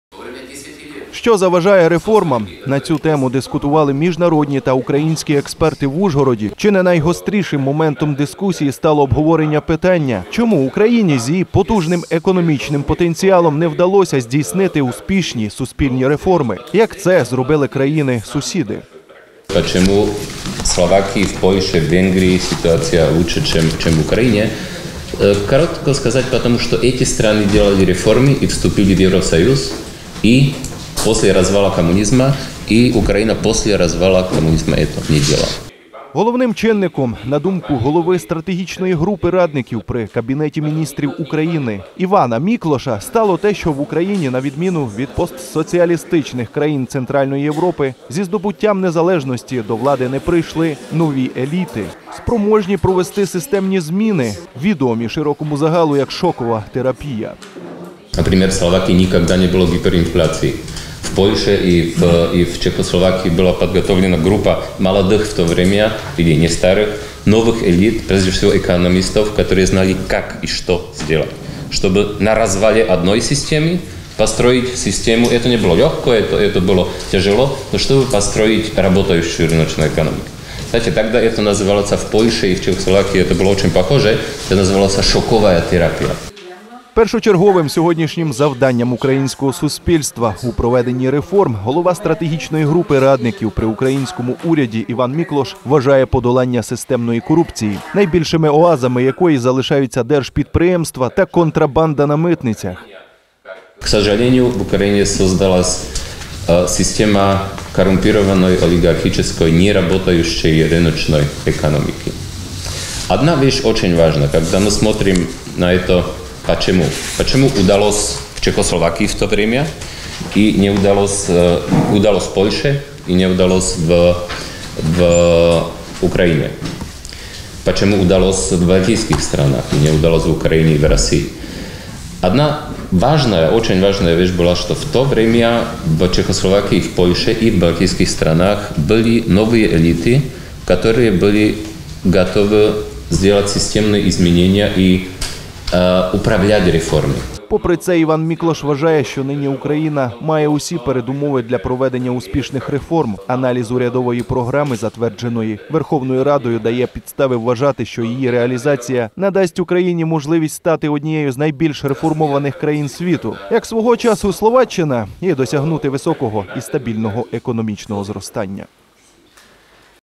Екс-міністр фінансів Словаччини поділився з ужгородцями досвідом успішних реформ
В Ужгороді дискутували міжнародні та українські експерти. Півсотні представників влади, науки, бізнесу, громадськості, дипломатичного корпусу, ЗМІ зібралися, аби перейняти досвід Словаччини, про який розповів головний реформатор країни-сусідки Іван Міклош. Під час обговорення йшлося про перебіг реформ в Україні: як вони проходять зараз і як могли би втілюватися за певних умов. Словацькі гості поділилися досвідом реформ у своїй країні, зокрема в інвестиційній політиці.